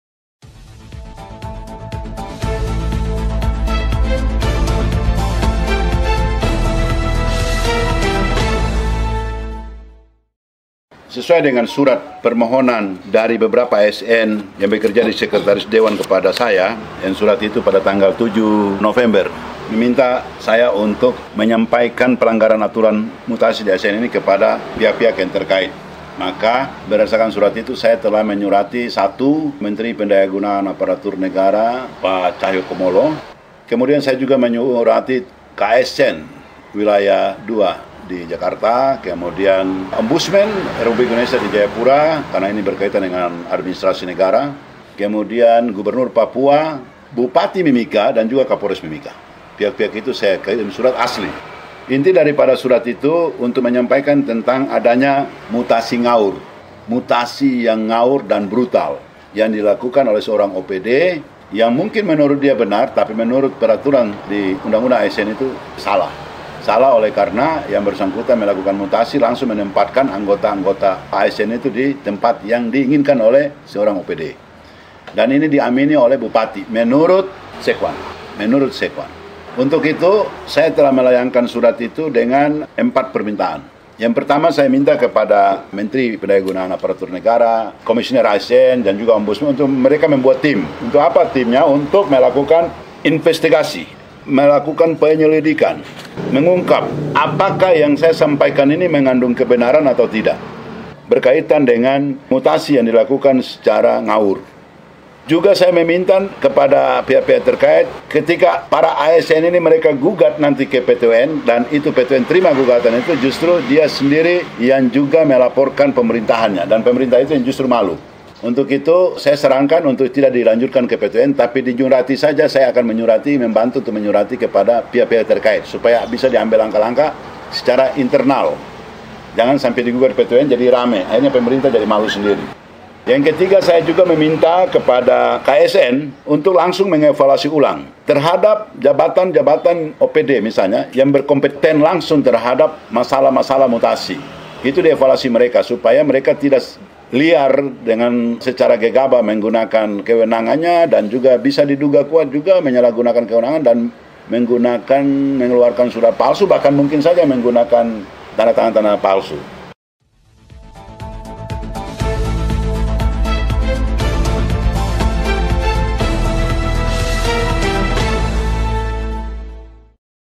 Diduga Ada Mutasi ASN di Lingkup Sekretariat Dewan Ngawur dan Brutal | DPRD Mimika Saleh Al-hamid
Legislator Sekretaris Komisi C DPRD Mimika Saleh Al-hamid telah menyurati sejumlah pihak termasuk pemerintah pusat terkait adanya dugaan pelanggaran peraturan dalam mutasi ASN khususnya di lingkungan Sekretariat Dewan DPRD Mimika.

Di kediamannya 18 November, Saleh mengutarakan ia telah melayangkan surat itu beserta dokumen bukti pendukung termasuk transkrip keterangan para saksi. Surat itu dilayangkan kepada Kementerian Pendayagunaan Aparatur Negara dan Reformasi Birokrasi, Komisi Aparatur Sipil Negara (KASN), Ombudsman RI, Gubernur Papua, Bupati Mimika dan Kapolres Mimika. Ia menilai mutasi itu asal-asalan atau ngawur dan juga sewenang-wenang alias brutal.

Empat poin permintaan dalam suratnya itu di antaranya : pertama ia meminta Kemenpan-RB dan KASN agar membentuk tim investigasi untuk menguji kebenaran suratnya.

Kedua, penyelesaian masalah mutasi dengan alasan sabotase itu diminta segera selesai tanpa membawa ke ranah hukum PTUN karena akan menjatuhkan wibawa Pemerintah Daerah Kabupaten Mimika. Penyelesaiannya diminta segera dilakukan secara internal.

Selanjutnya KASN diminta untuk mengevaluasi para OPD di Mimika yang berkewenangan dalam hal mutasi ASN, sehingga dugaan pelanggaran aturan tidak selalu terjadi ke depannya.

Poin empat, Polres Mimika diminta menindak-lanjuti laporan para ASN terkait dugaan pencemaran nama baik karena mutasi dengan alasan ‘sabotase’ itu.

Saleh Al-hamid Legislator asal Partai Hanura itu menambahkan, aduan 14 ASN yang mempercayakan nasibnya untuk diperjuangkannya itu diminta agar ditindak-lanjuti dalam waktu yang tidak terlalu lama sehingga dapat kembali bekerja tanpa beban dan kembali menjalin hubungan yang harmonis di lingkup Sekretariat Dewan DPRD Mimika.(VB)